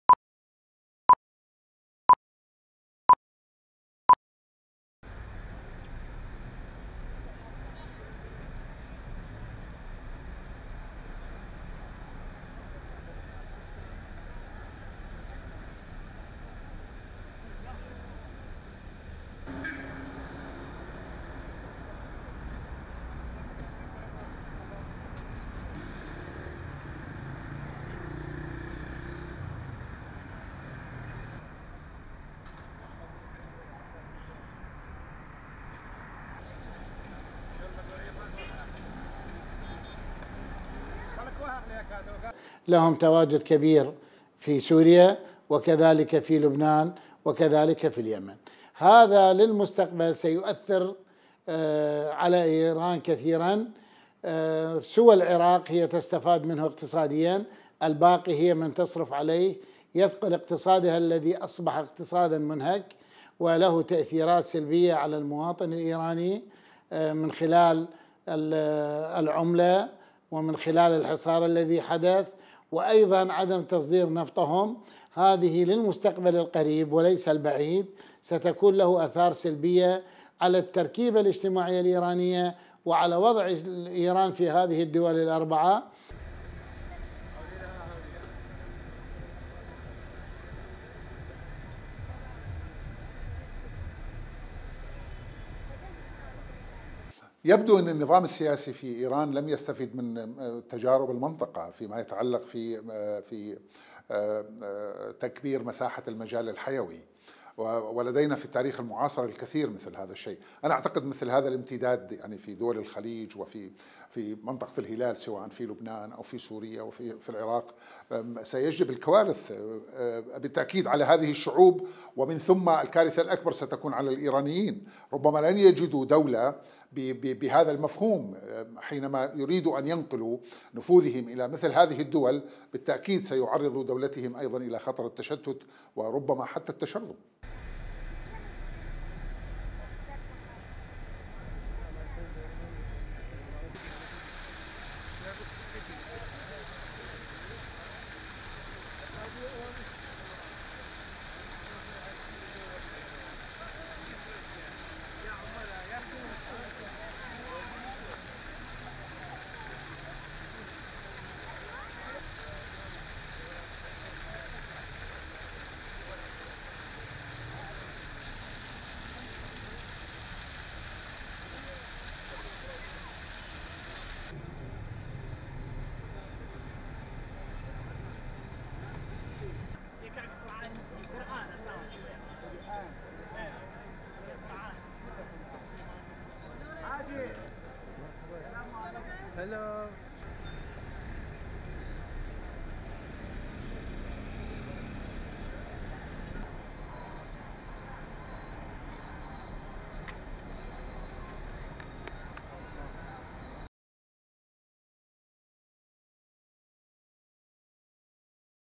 Iraq |Analysts: The Intervention In Syria, Lebanon, Yemen And Iraq Has Exhausted Iran Economically
Analysts considered that the Iranian intervention in many Arab countries, such as Syria, Lebanon, Yemen and Iraq, has exhausted Iran economically until its effects became obvious inside Iran through the decline in the exchange rate of the toman against the dollar due to the economic blockade imposed on Iran, which prevents it from exporting oil, they pointed out that the Iranian regime has not learned from the experiences that have taken place in the Middle East from the many attempts made by various regimes to enlarge their vital area, all of which failed, they also stressed that these interventions will put the Iranians and their state at a risk of fragmentation.

العراق | محللون : التدخل في سوريا ولبنان واليمن والعراق أنهك إيران اقتصاديا

اعتبر محللون أن التدخل الإيراني في العديد من الدول العربية، مثل؛ سوريا ولبنان واليمن والعراق، أنهك إيران اقتصاديا حتى أصبحت آثاره بادية في الداخل الإيراني من خلال انخفاض سعر صرف التومان مقابل الدولار بسبب الحصار الاقتصادي المفروض عليهم والذي يمنعهم من تصدير النفط، منوهين إلى أن النظام الإيراني لم يستفد من التجارب التي حدثت سابقا في منطقة الشرق الأوسط من محاولات لتكبير منطقة المجال الحيوي لعدد من الأنظمة والتي باءت جميعها بالفشل، كما شددوا على أن هذه التدخلات ستعرض الإيرانيين ودولتهم لخطر التشتت والتشرد.